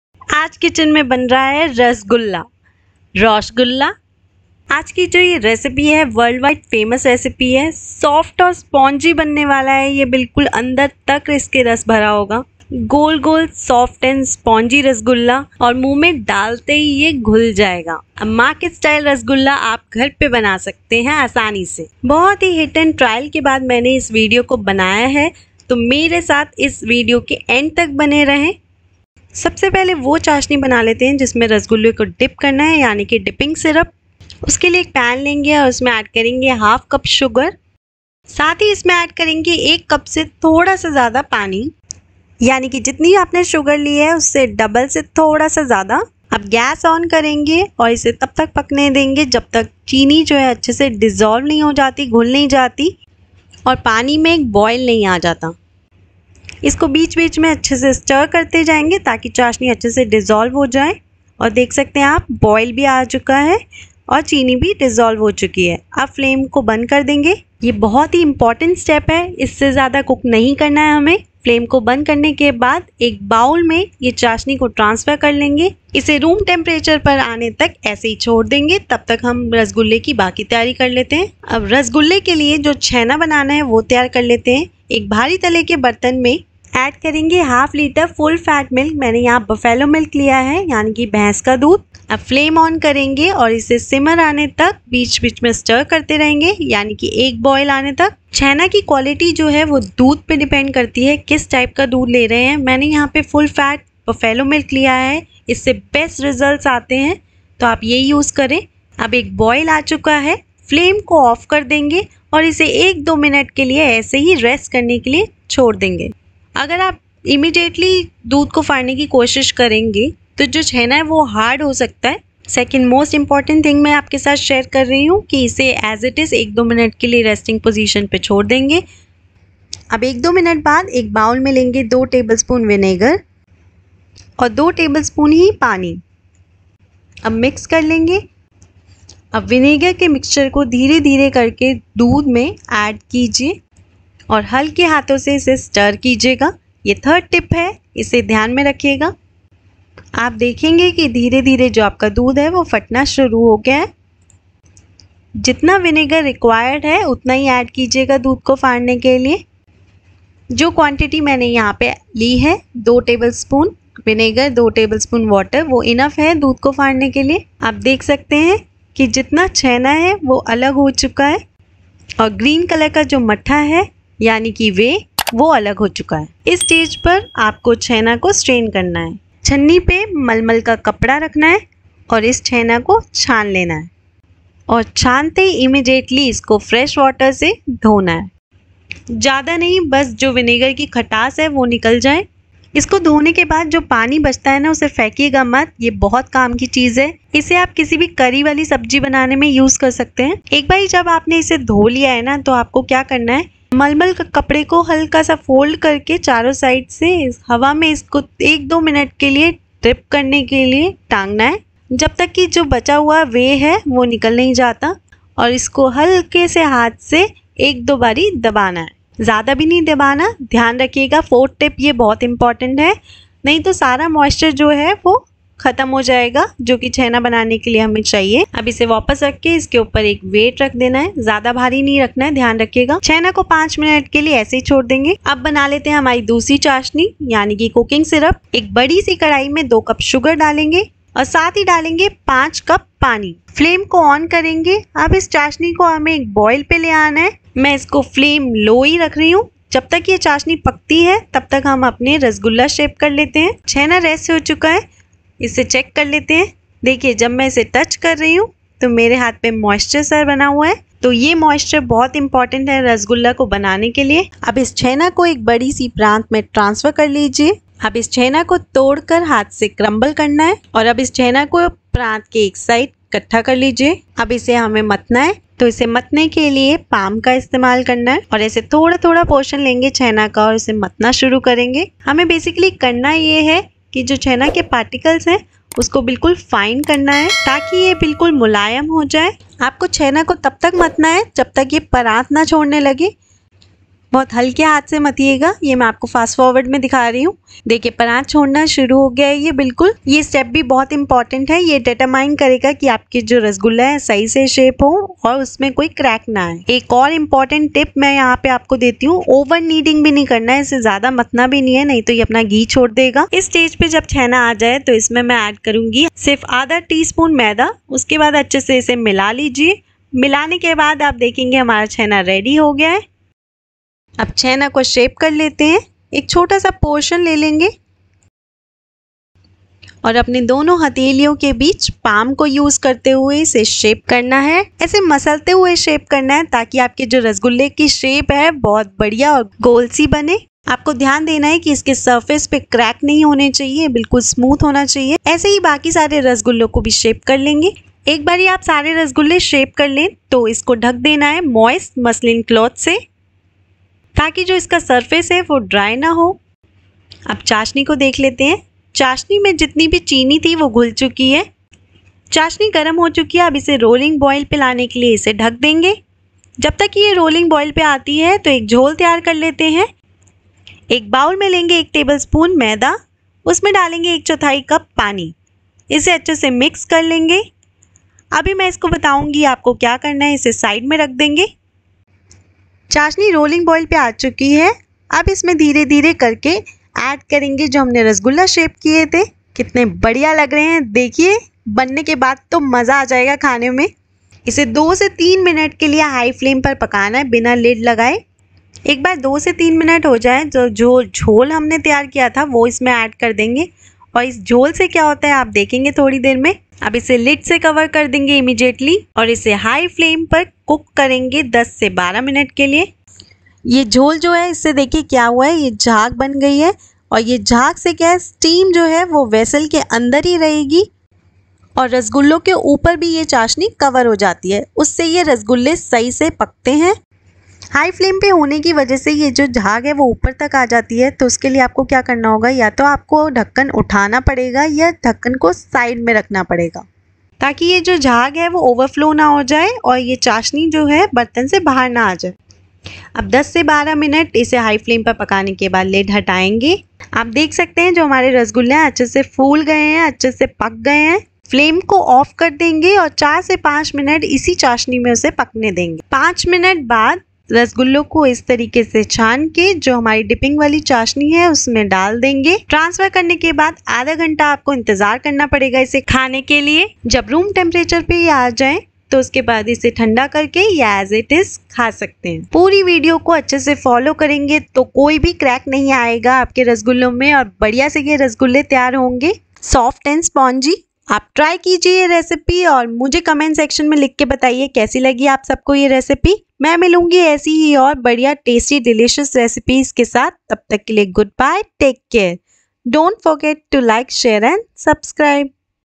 100% स्वादिष्ट बाजार जैसे स्पंजी फूले फूले पर्फ़ेक्ट नरम रसगुल्ले | Rasgulla Recipe | SG FOOD WORLD
Hello friends , iss video mai mein aapke saath share karungi kaise asaan tarike se 100% स्वादिष्ट बाजार जैसे स्पंजी फूले  फूले पर्फ़ेक्ट नरम रसगुल्ल्ले बनायें | Recipe ko poori dekhein aur sahi tarike se extra spongy rasgulla ghar par banaein. Video mein maine saari tips & tricks share ki hain.

Ingredients:
   Dipping syrup
• SUGAR | शक्कर 1/2 CUP / 100 GRAMS
• WATER | पानी 1 CUPS + 1/4 CUP

• MILK | दूध 1/2 LITRE (FULL FAT)
• VINEGAR | सिरका 2 TBSP
• WATER | पानी 2 TBSP

• Cooking syrup
• SUGAR | शक्कर 2 CUP / 400 GRAMS
• WATER | पानी 5 CUPS
• REFINED FLOUR | मैदा 1/2 TSP
• REFINED FLOUR | मैदा 1 TBSP